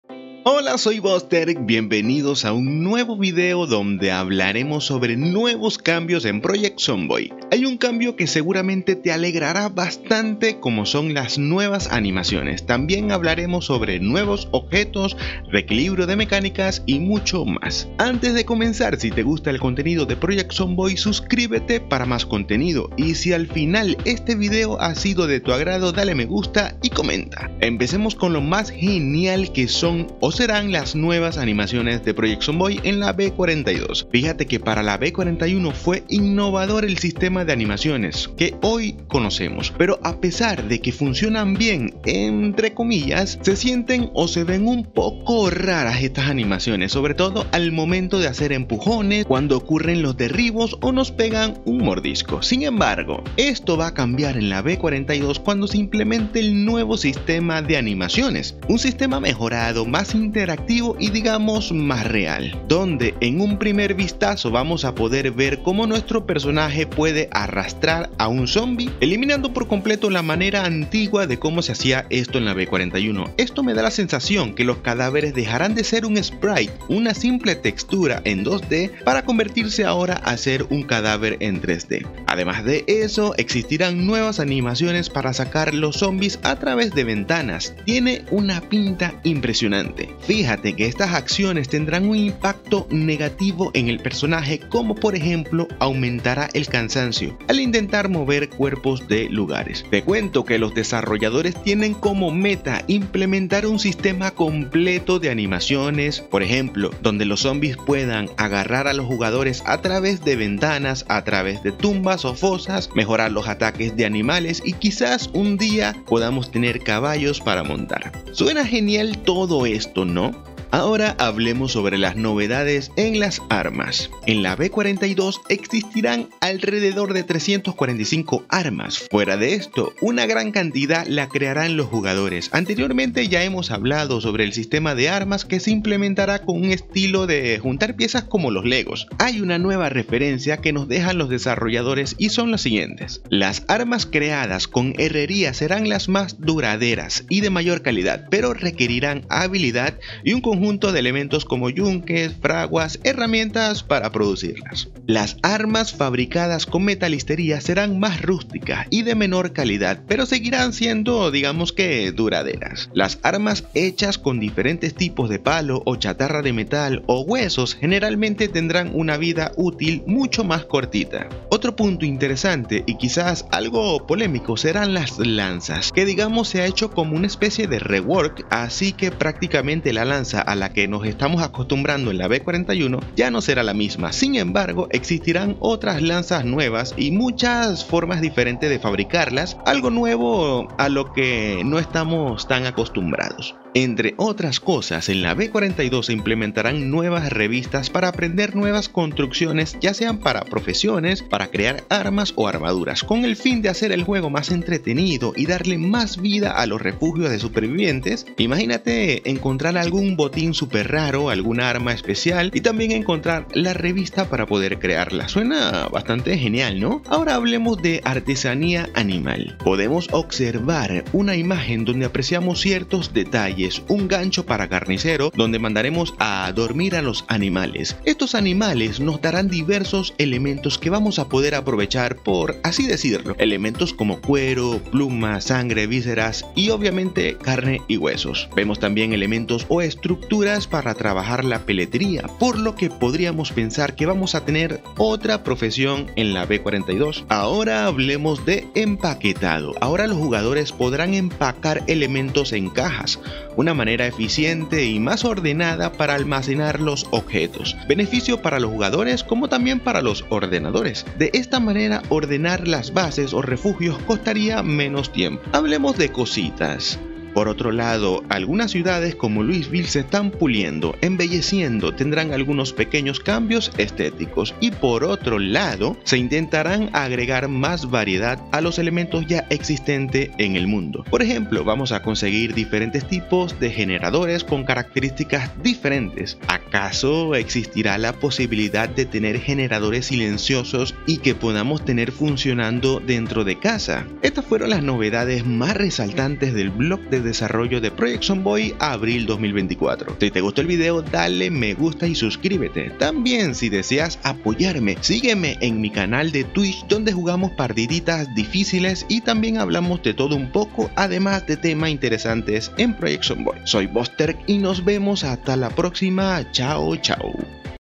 ✅ ANIMACIONES MAS REALISTAS! NUEVAS NOTICIAS PROJECT ZOMBOID BUILD 42 - ABRIL 2024
🔥Nuevas noticias del juego PROJECT ZOMBOID b42, en este video te cuento las ultimas novedades para la tan esperada version B42, te hablare sobre temas de:
Nuevas animaciones
Nuevos items
Nuevas mecanicas
Reequilibrio de armas cuerpo a cuerpo

nos preguntamos cuando saldra la build 42 de project zomboid?

Si deseas leer el blog de noticias de desarrollo del juego project zomboid: